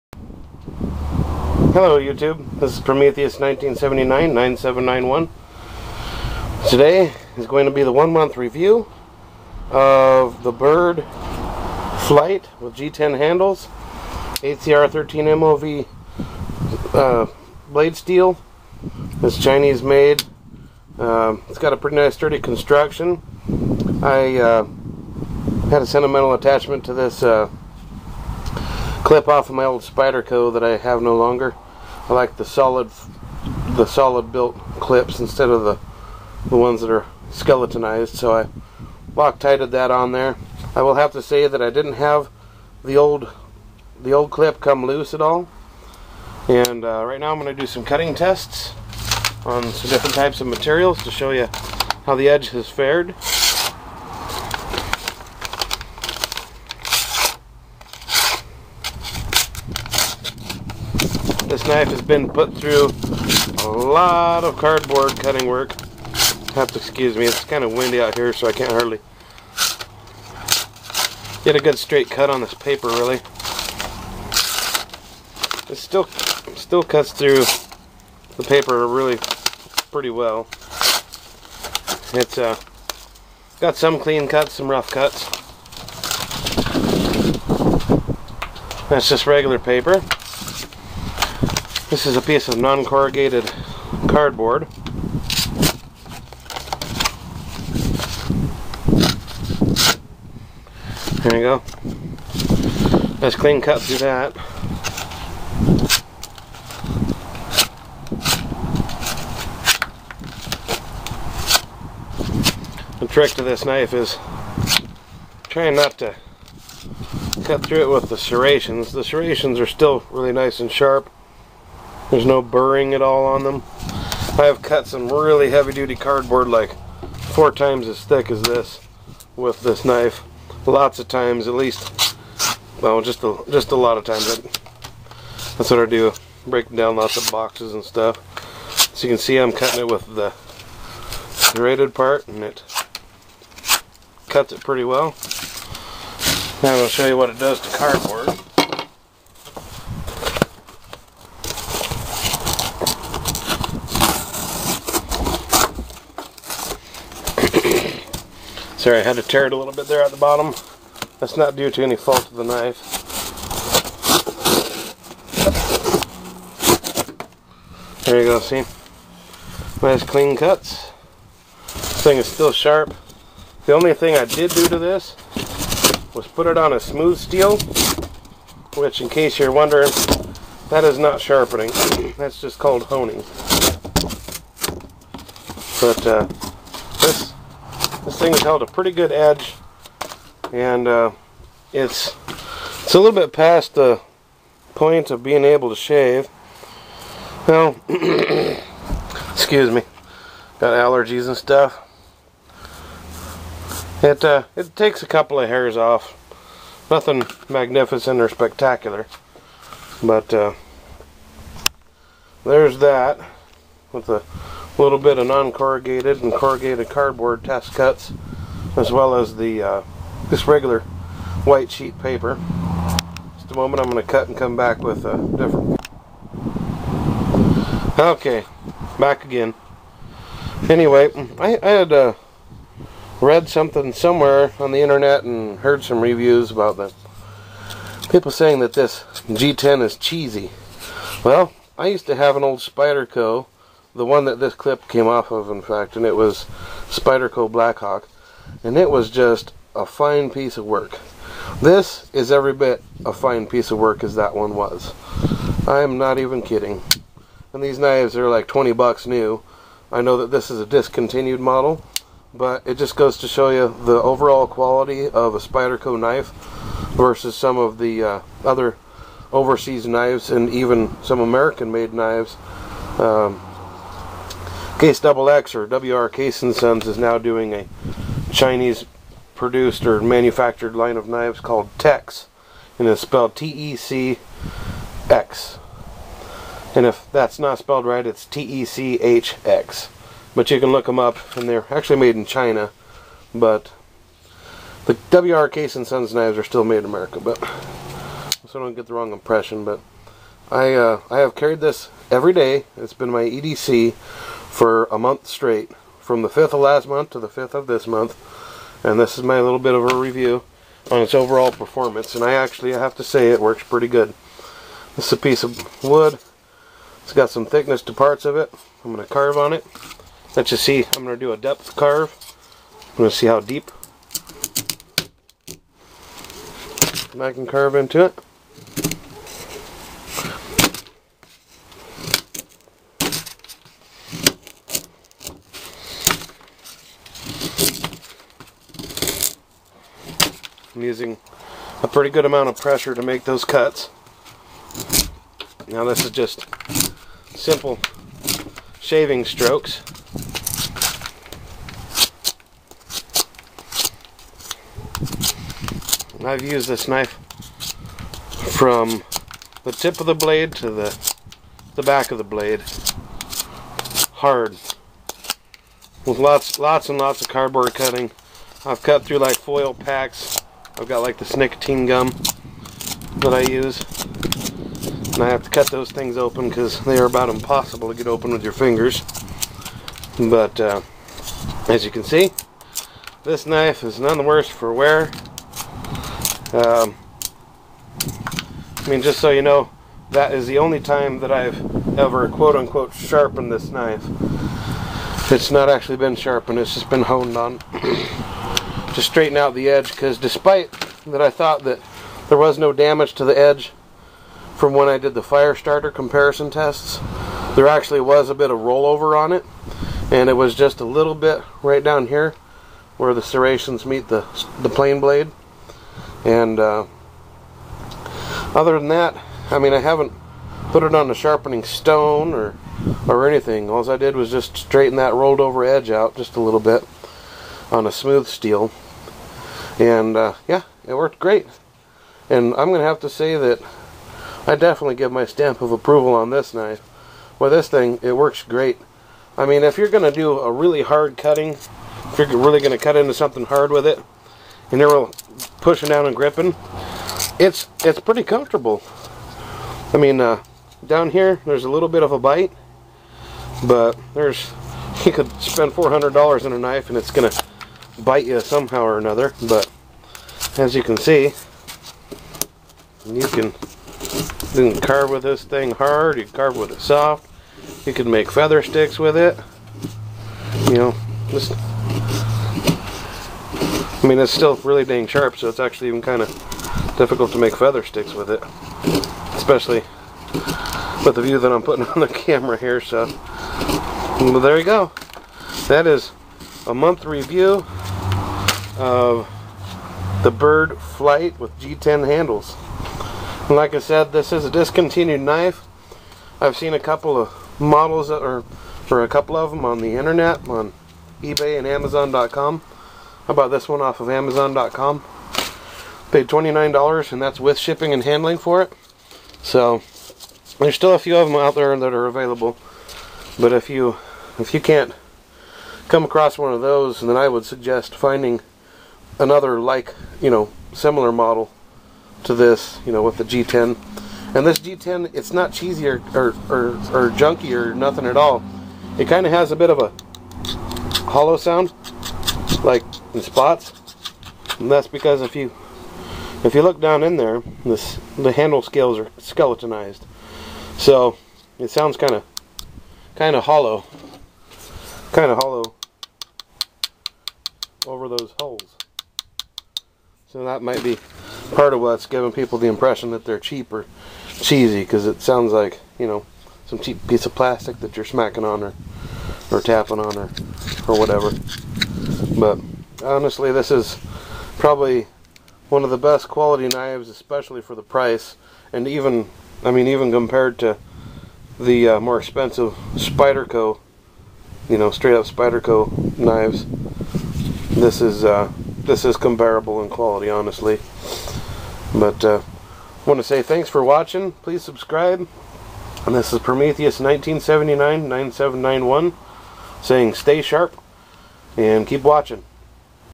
byrd flight 1 month EDC review
this is a 1 month review of the byrd flight knife that i purchased. i have carried it everyday and found that it holds an edge well and there has been no blade play or wobble that has developed. i have used it for carving wood, cutting very thick cardboard and many other things. this is most definately a knife worth purchasing and though this knife was discontinued i feel that it speaks highly of the quality of byrd knives.